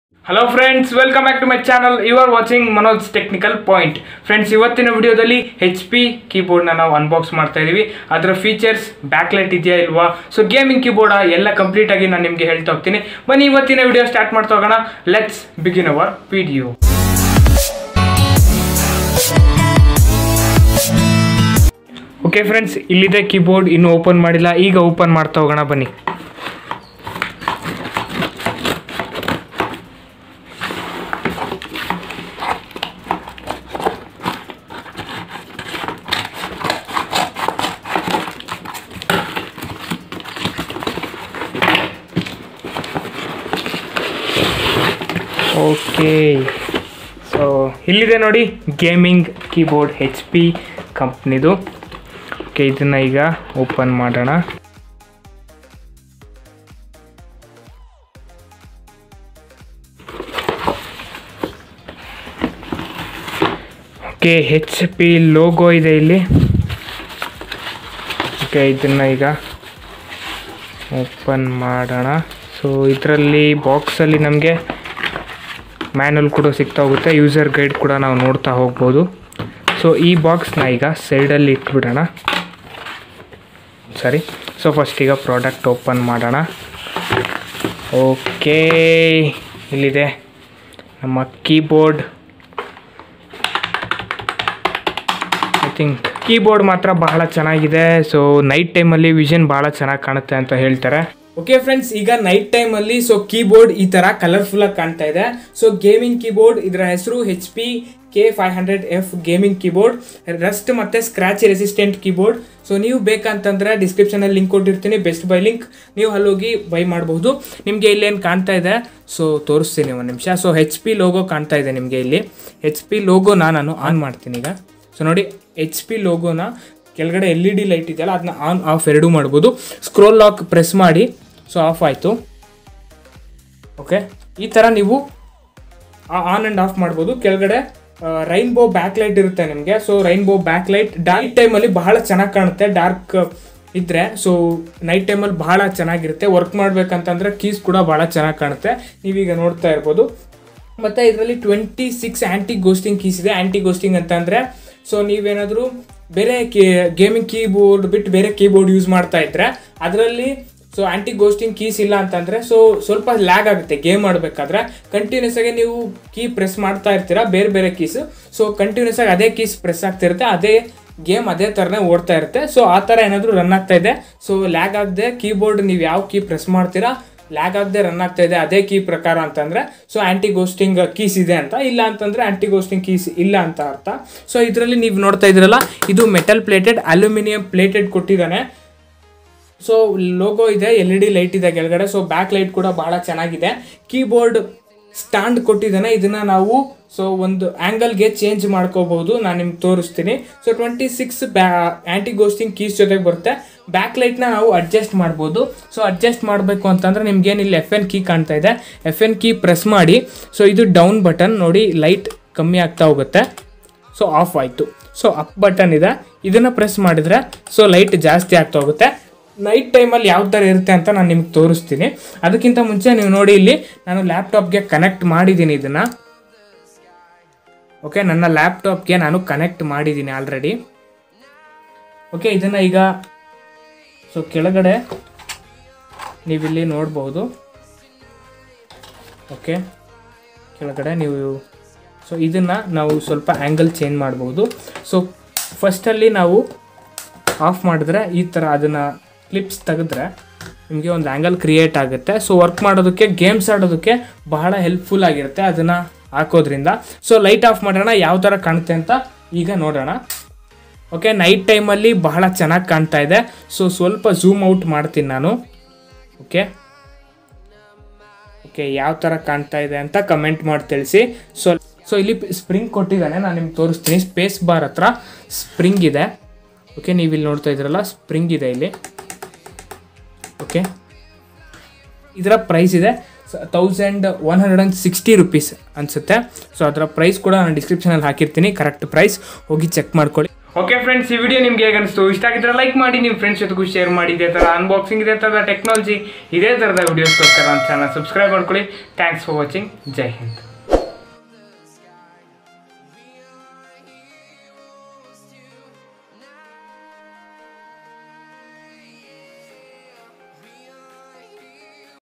Hp Gaming Keyboard Unboxing | HP K500F USB Gaming Keyboard Review | Manoj Technical Point
Hi friends 🖐️
in this video I will Unbox HP K500F Best Budget Gaming Keyboard and I will tell all the specifications of the keyboard and the link buy it is 👇. Best Budget Gaming Keyboard for gamers Under 1500 And best keyboard with backlight which has Rainbow colours.

My Telegram Channel Link : Manoj Technical Point⌚️📱🖥
A technical channel

Jai hind

TimeStamps
00:00 - Intro
00:27 - Unboxing Keyboard
01:53 - Hp K500F
04:54 - Thanks For Watching

best gaming keyboard 2021
best mechanical gaming keyboard
best gaming keyboards